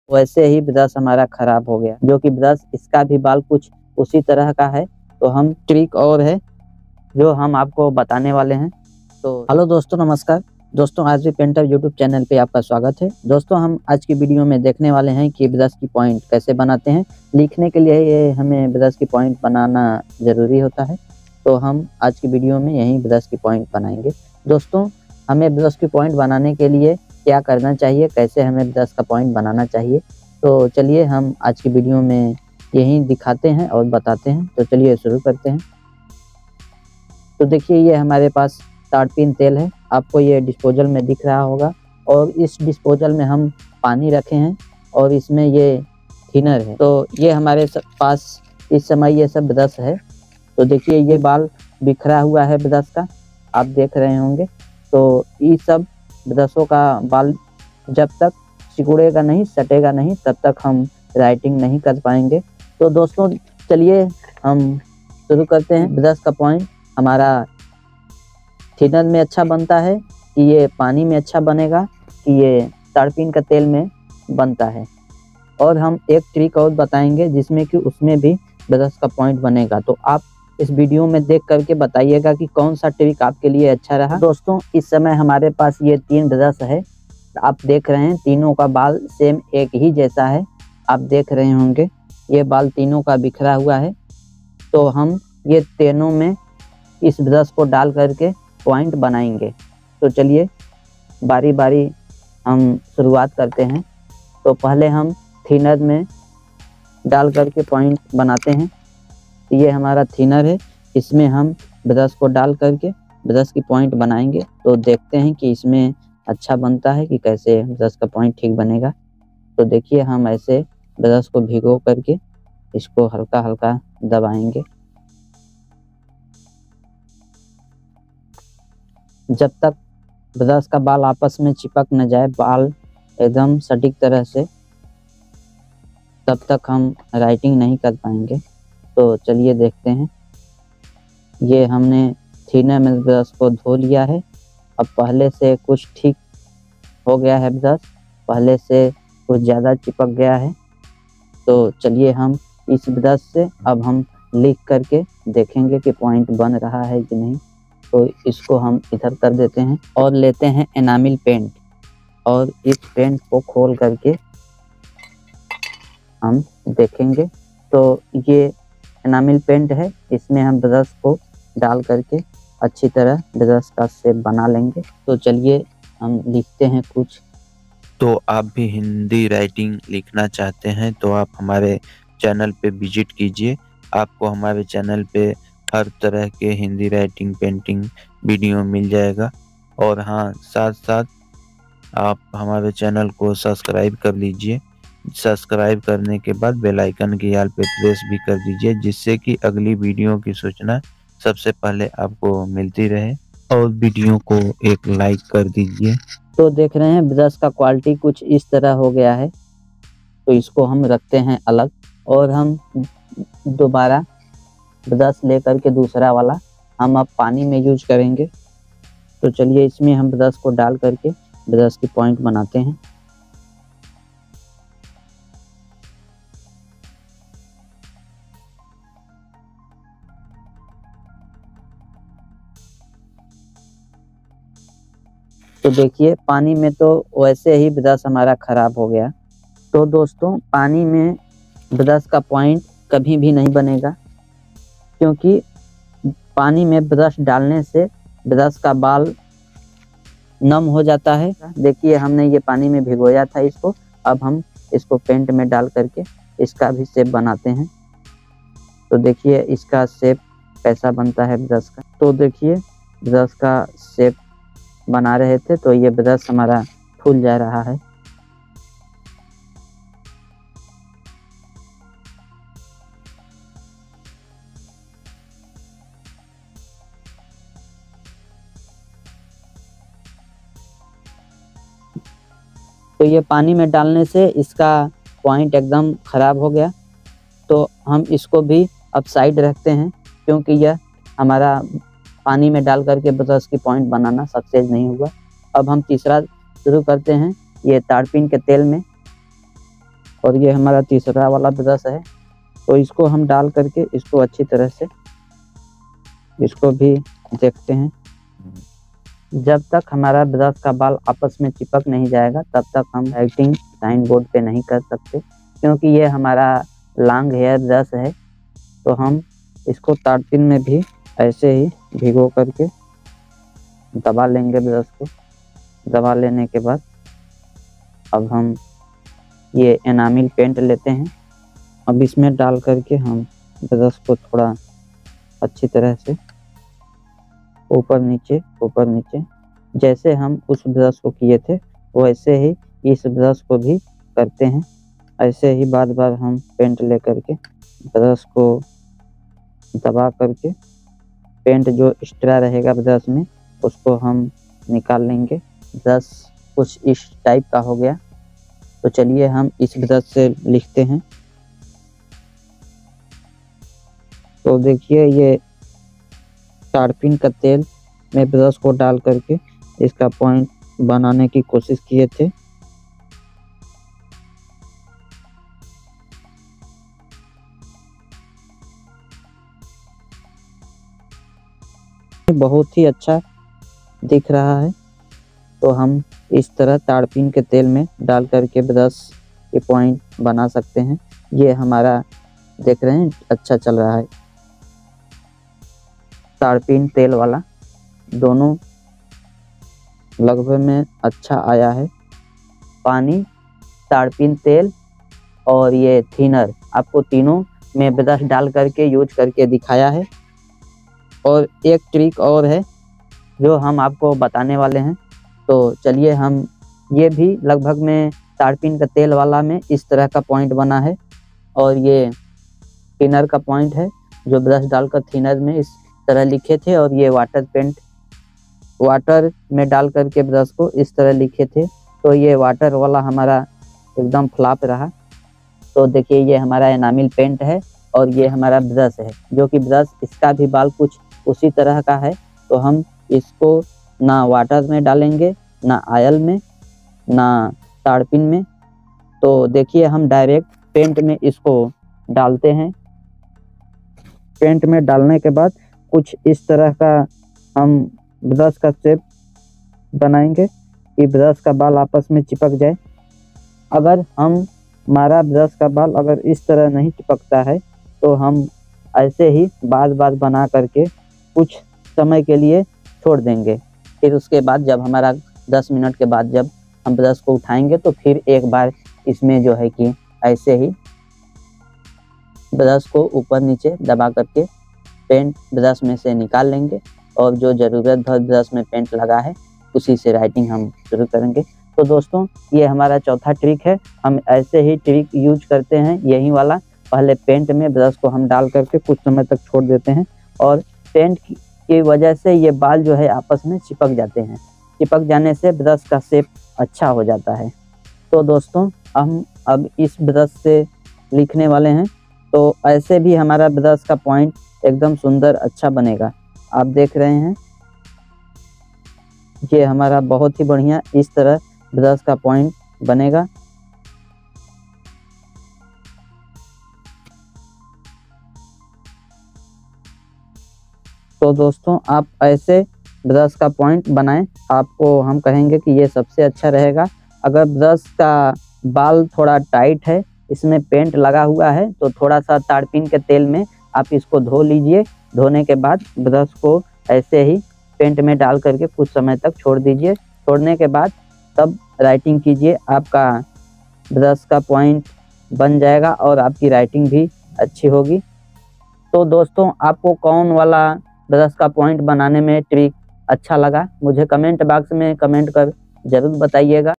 Writing Brush Point/राइटिंग करने से पहले ब्रश की प्वाइंट कैसे बनाएं।
2" Arora synthetic writing painting-: brush
about this video -: In this video we have given the information about brass.  How to make brush points before painting with a brush.  If the hair of the brush is open, then it is necessary to make a point.  Brush point oil paint like oil, thinner and water are described in all three ways. So by watching this video you understand how to make brush point. At the end we have made brush point with paint also. So in this video you will get  You will get complete information about brush points.